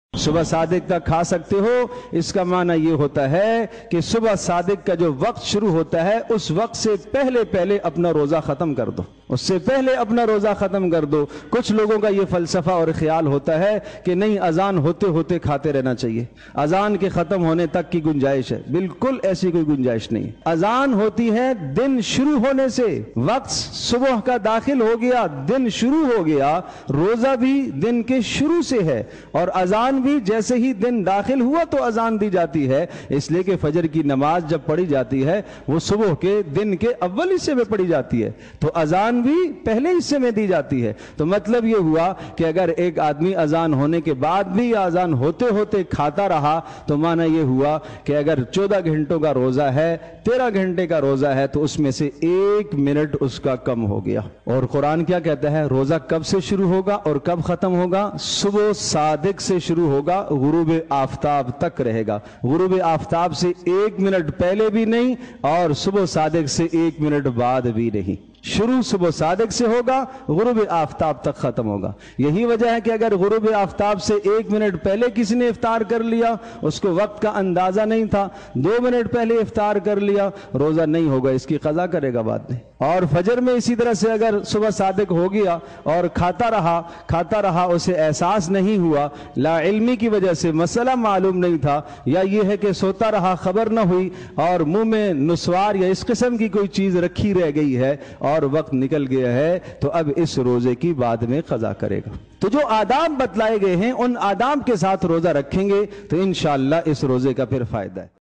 Sehar o iftar kay Auqaat kay Mutaliq aik bohat bari Galat Fehmi Maulana Manzoor Yousuf
سحرو افطارکے اوقات کے متعلق ایک بہت بڑی غلط فہمی

حضرت مولانا منظور یوسف صاحب دامت برکاتہم العالیہ